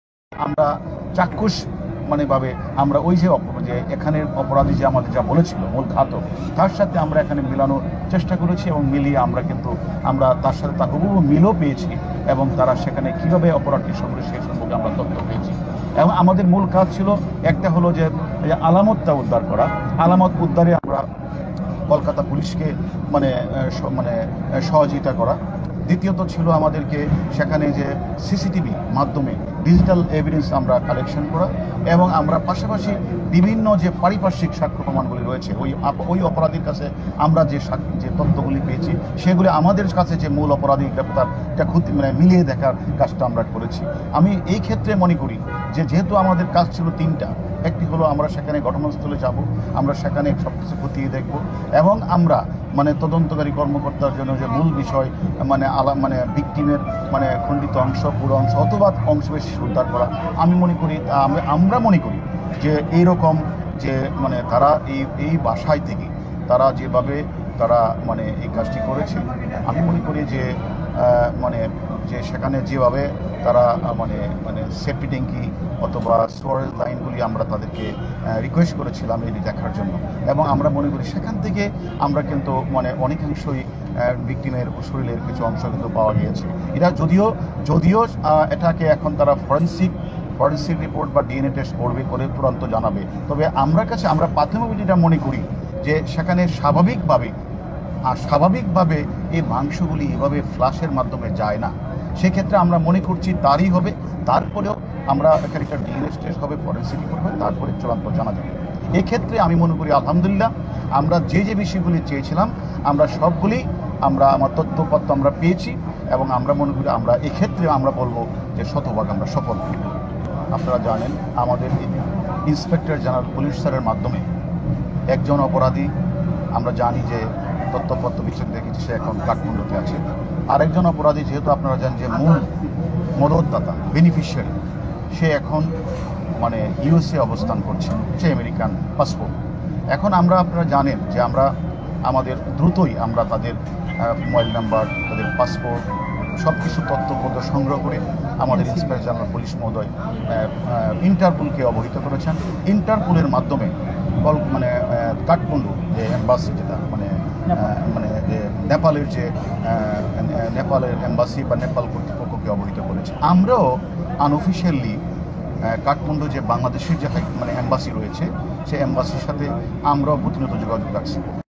‘ওয়াটার থিওরি’ অনুসরণ করেই এমপি আনার হত্যাকাণ্ডের রহস্য উদঘাটনে সফলতা এসেছে
ধন্যবাদ এতক্ষণ সঙ্গে থাকার জন্য।